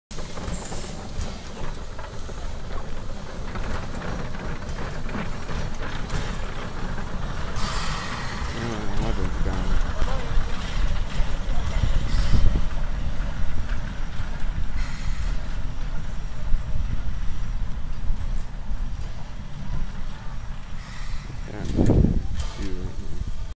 Cass Scenic Railroad shay 11 pulls away from the Bald Knob train in Bald Knob West Virginia
They used 2 shays to get up the mountain and 1 to get down. 11 and 2 pushed us up and 2 pulled us down. 11 is heading back to Cass.

5/29/21